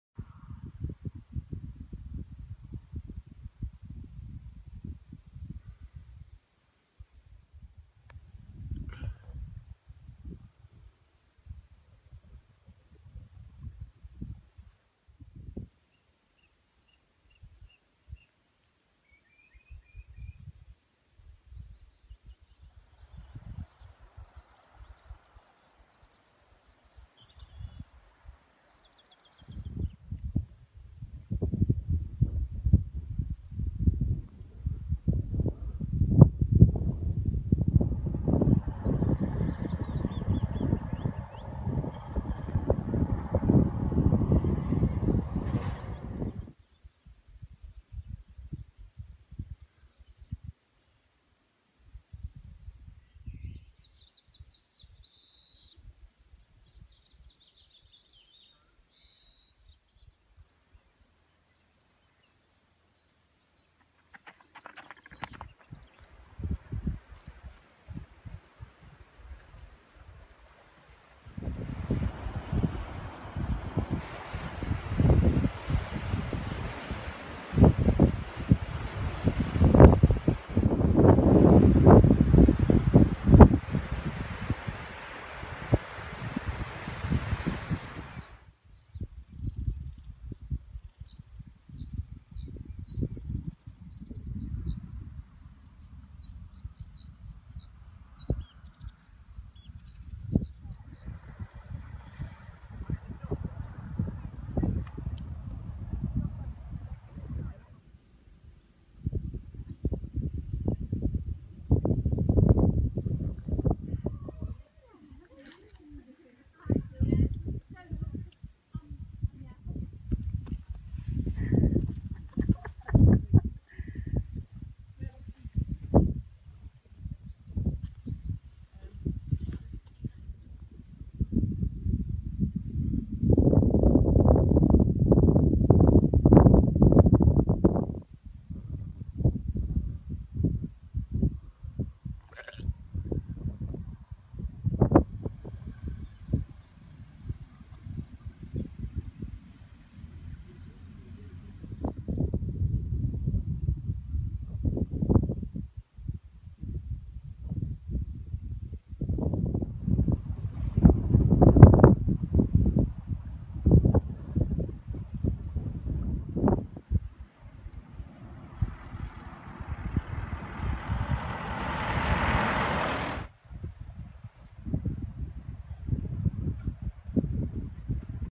Day 10 Scourie - Rue
Monday 23rd May 2016
Scourie - Rhue 65 miles
A894 Kylestrome - Kylesku - Unapool
B869 Ardvar - Nedd - Drumbeg - Clashnessie - Balchladich - Stoer - Clachtoll
A837 Little Assynt - alongside loch Assynt  - ardvreck castle - Inchnadamph - Stronechrubie - Ledbeg - Ledmore
A835 Elphin - Drumrunie - Strathcanaird - Ardmair - Rhue